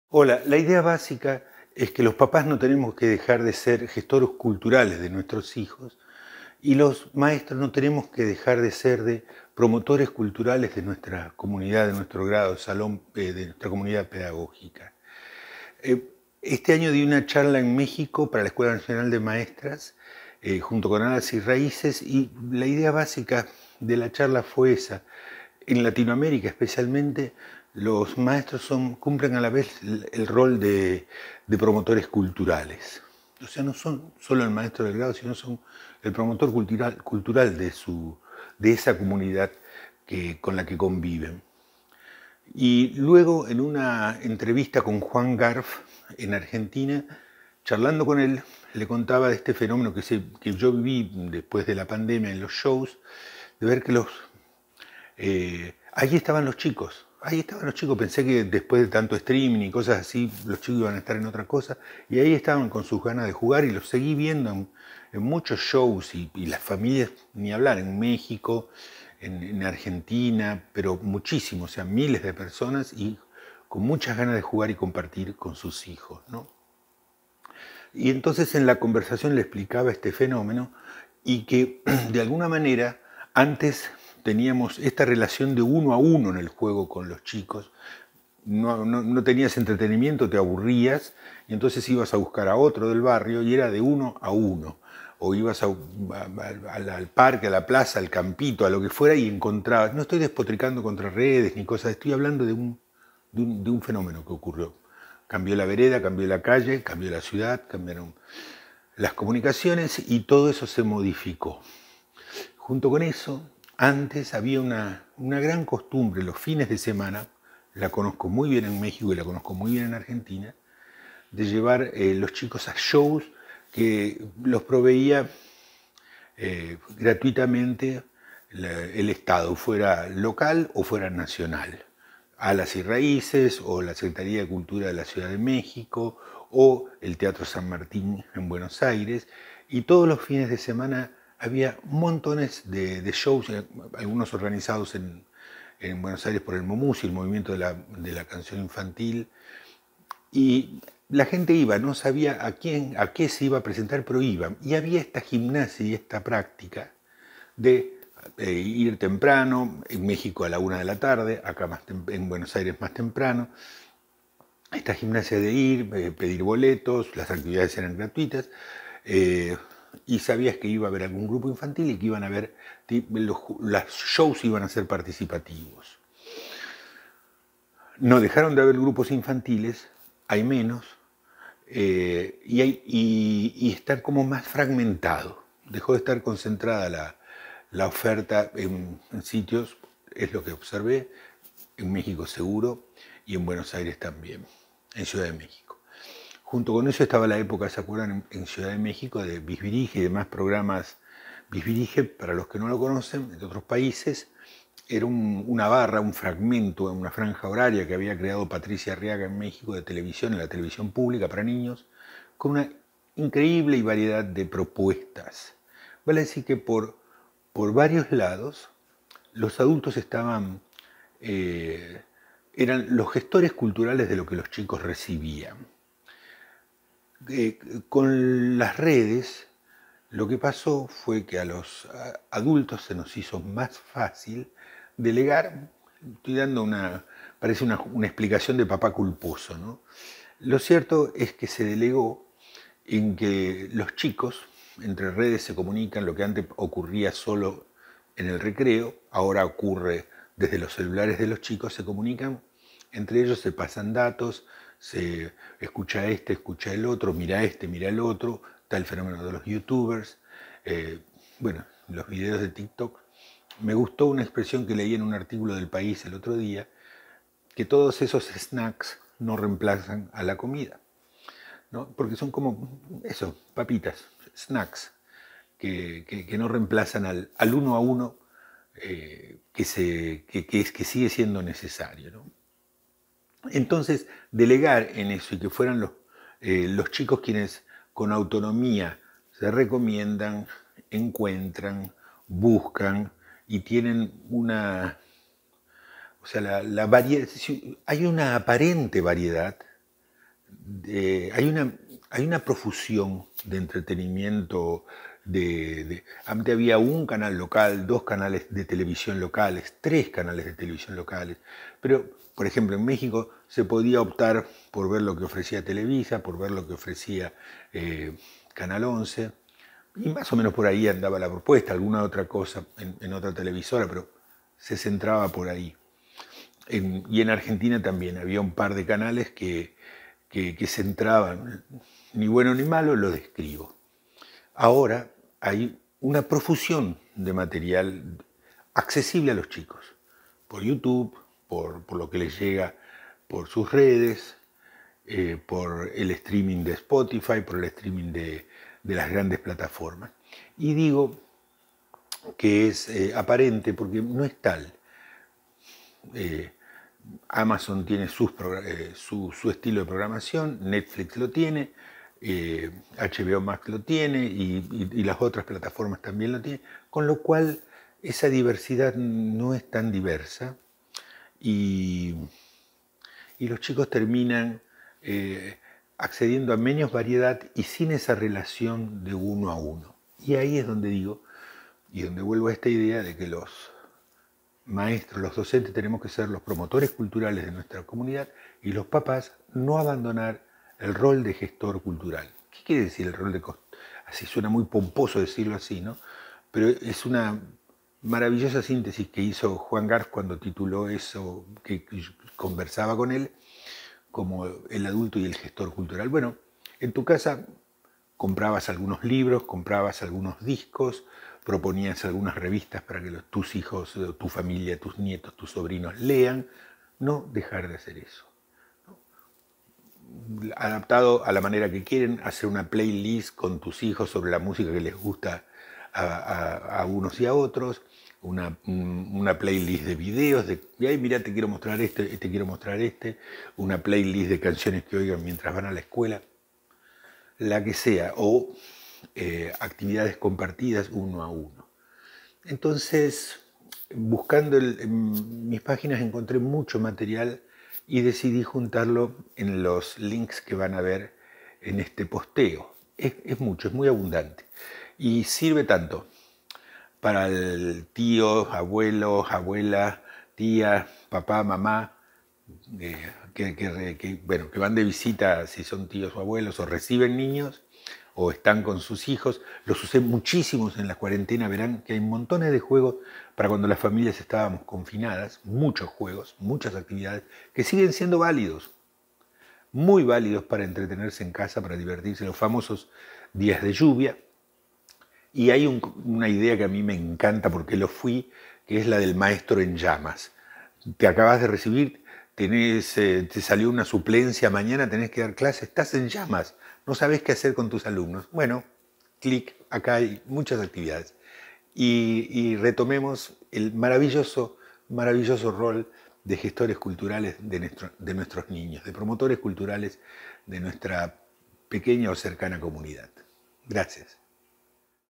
Los adultos como gestores culturales de nuestros chicos
Los padres no tenemos que abandonar nuestro rol de gestor cultural de nuestros hijos (sobrinos, nietos): en los libros, la música, películas, juegos que les buscamos. No delegar ese rol en lo que ellos buscan por sí solos en redes, o los que les llega por streaming. Seguimos siendo el canal privilegiado porque somos el único espacio en una relación uno a uno: en casa, en el club, la escuela, los parques.
* Si empiezan a dar clases mañana y están en llamas pues no se les ocurre qué hacer
* Por si llevamos días en casa, como ocurrió durante el confinamiento, y ya no tenemos juegos en la galera
* Por si recibimos sobrinos, nietos...
... Acá hay muchas canciones, juegos y actividades de animación cultural
Dar vuelta el presente con juegos y canciones: